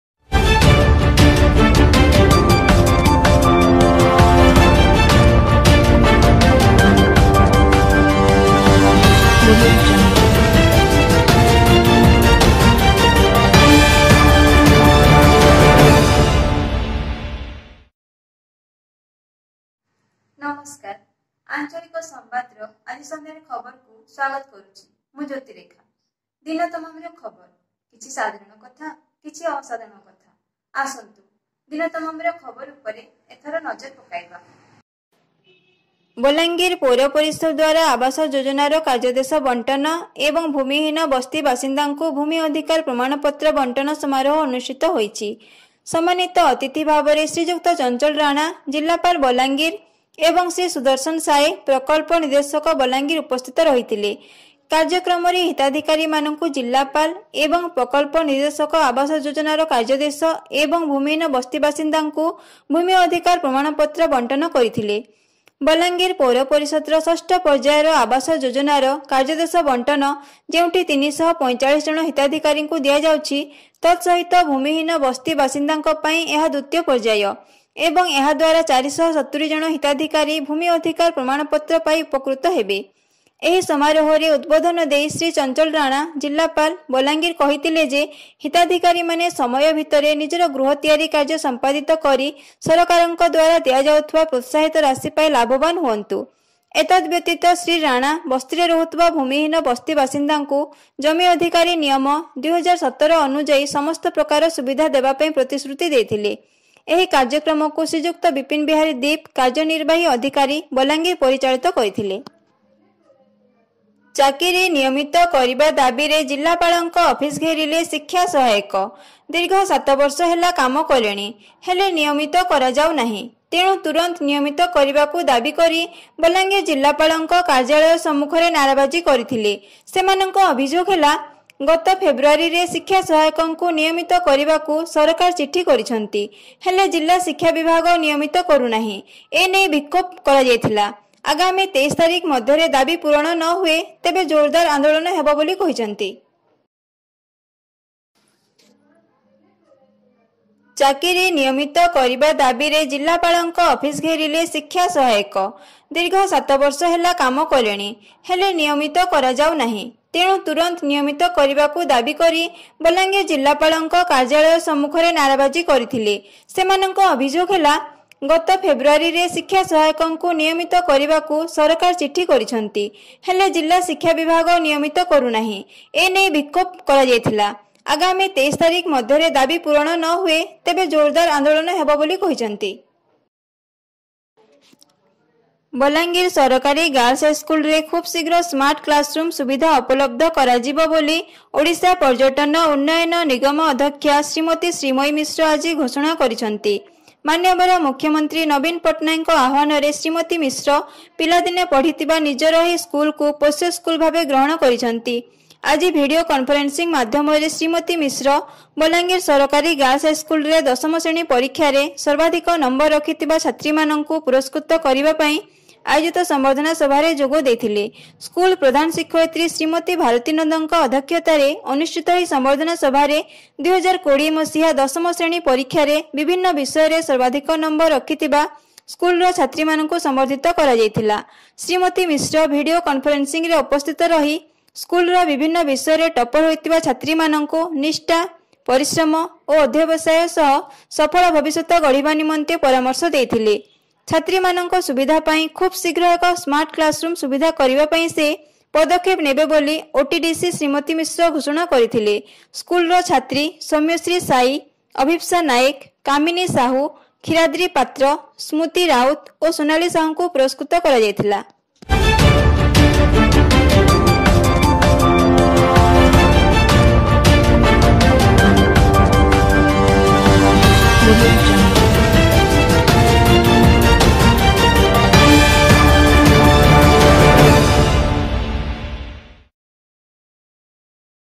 Evening news Dt 25.03.2021
news on
workorder distributed by collector to beneficiary of PMAY
demonstration by sikhya sahayak for permanent posting
COVID19 SITUATION
develipment of govt girls high school. bolangir